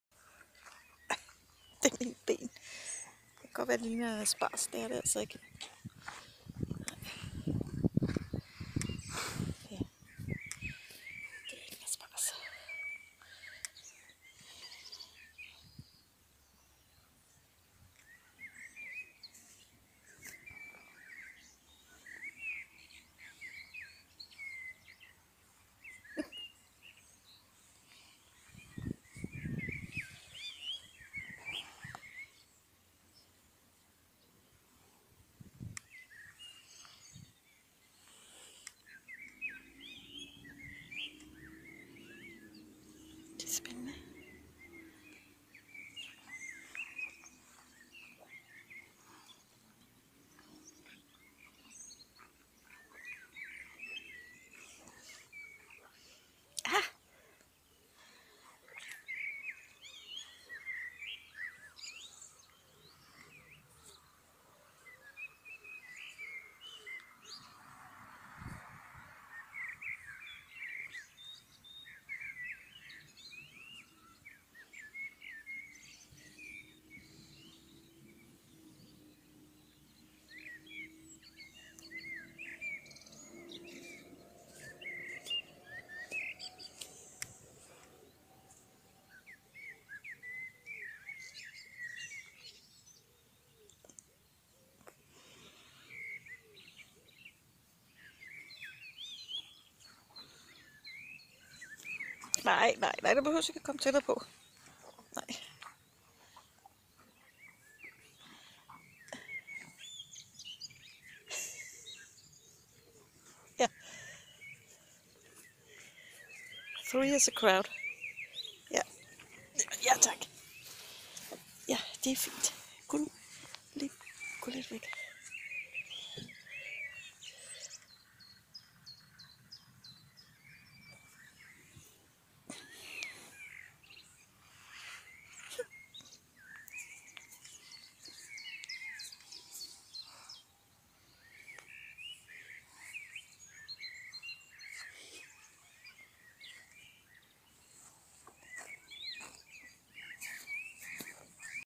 Sommerferiedrømme
Behold i bare jeres tur til Thailand og til Sunny beach. Behold All inklusive og safarier og lange ventetider i lufthavnen. Når jeg får ferie, så bliver jeg lige her på et tæppe midt i mit Paradis. Der er intet sted jeg hellere vil holde ferie!